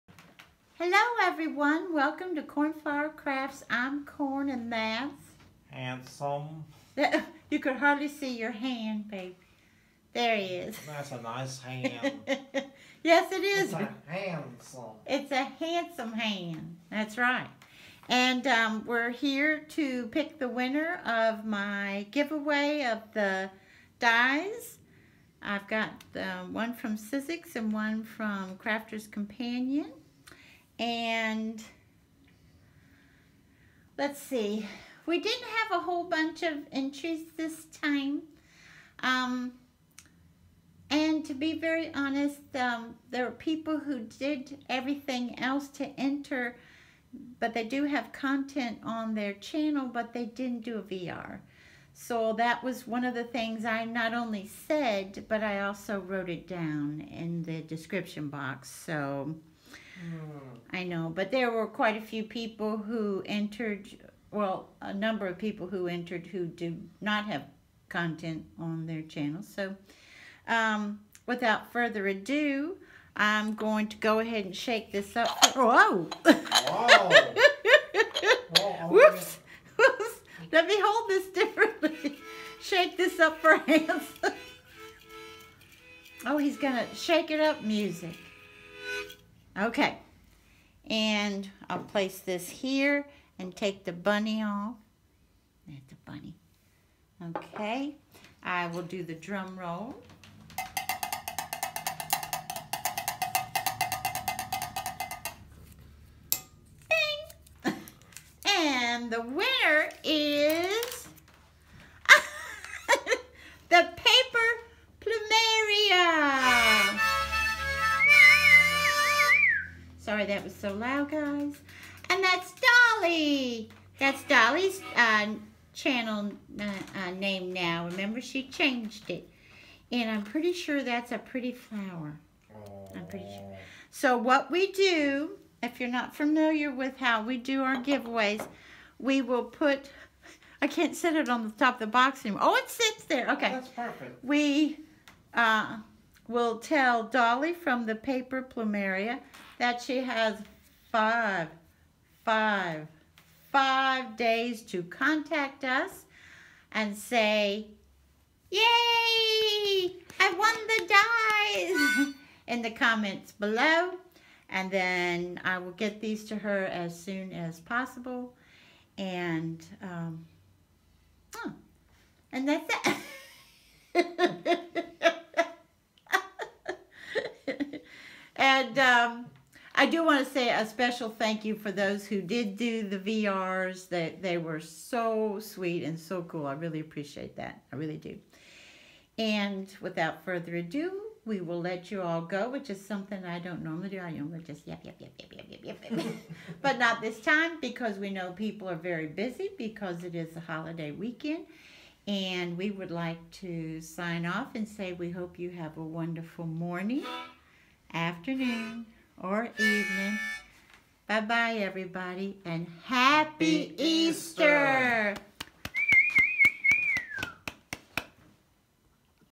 Winner of Metal Dies Announced
THIS VIDEO IS INTENDED FOR PERSONS 18 YEARS OLD OR OLDER
Please excuse my nails, but I have been crafting.  Hello everyone!  Handsome picked the winner of my latest metal dies giveaway, and the winner is...